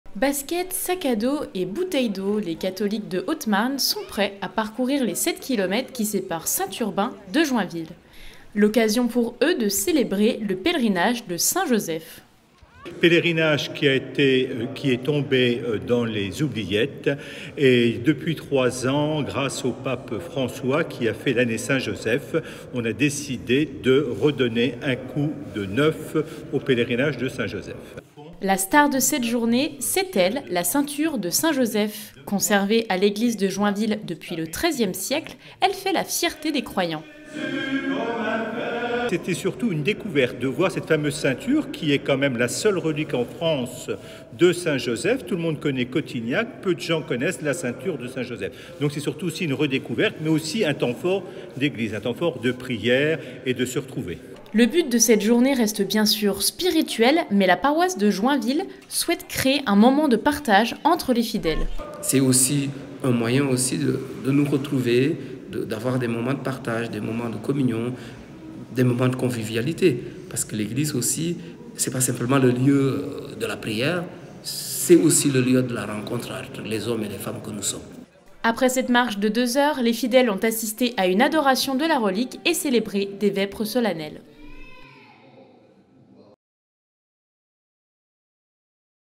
7 kilomètres pour honorer Saint-Joseph
Ce week-end, une cinquantaine de fidèles se sont retrouvés devant l’église du petit village de Saint-Urbain-Maconcourt pour célébrer le pèlerinage de Saint Joseph.